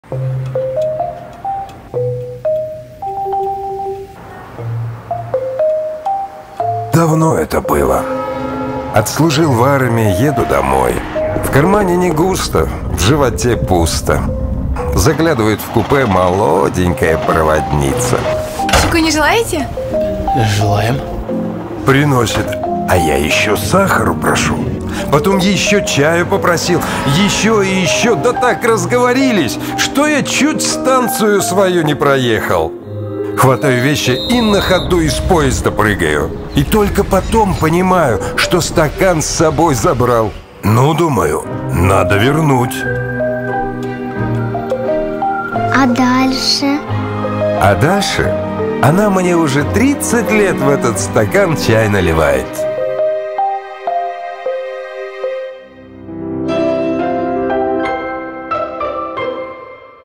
Реклама чая Пиала "Подстаканник"
Рекламный ролик для чая Piala Gold "Подстаканник".
Creative Director: Сергей Добрыднев
Creative Group Head: Евгений Костылев
Copywriter: Болат Элмуратов
Client Service Director: Юлия Тушина
Account Director: Дамеш Танашева
Агентство: GForce/Grey

RG Brands:
Marketing Director Food Category: Эльвира Болотова
Brand Manager: Эльзат Ахматов

Production:
Режиссер: Drago Sholev (Болгария)
Family Films (Казахстан) -
Директор: Татьяна Орлова
Продюсеры: Андрей и Анастасия Малаян, Константин Масленников
One Little Production (Белоруссия)